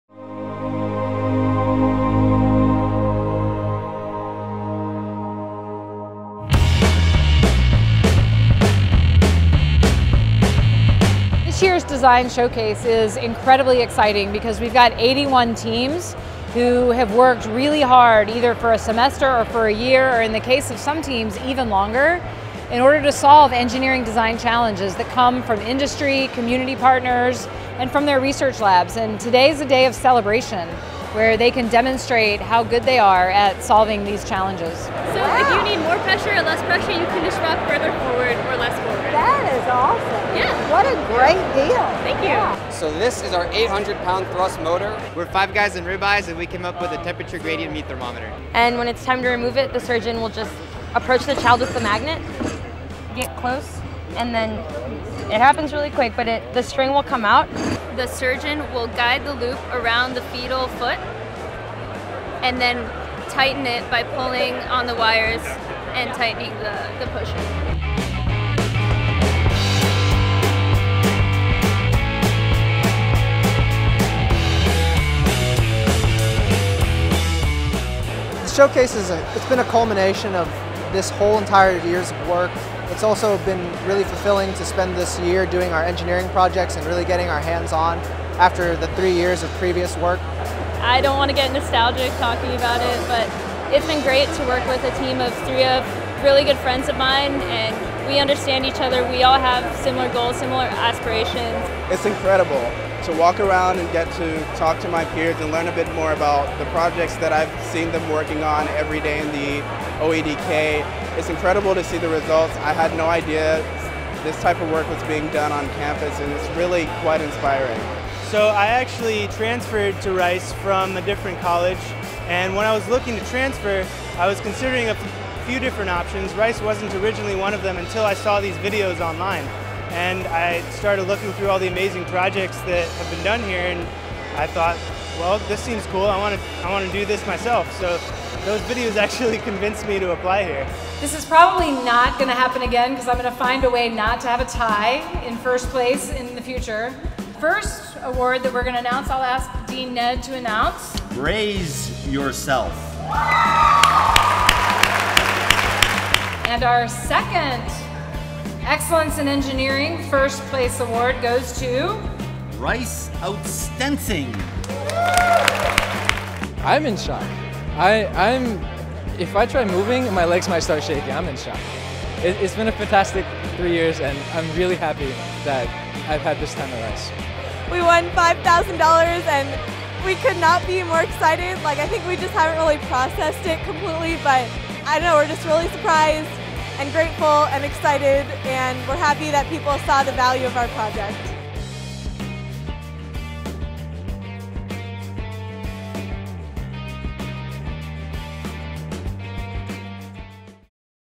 Brays Yourself and Rice Outstenting tie to win Engineering Design Showcase
For the first time, two senior engineering teams, Brays Yourself and Rice Outstenting, tied for the top prize in the George R. Brown Engineering Design Showcase, held April 14 at Rice University’s Tudor Fieldhouse. Each was granted the top prize of $5,000 for the Excellence in Engineering Award.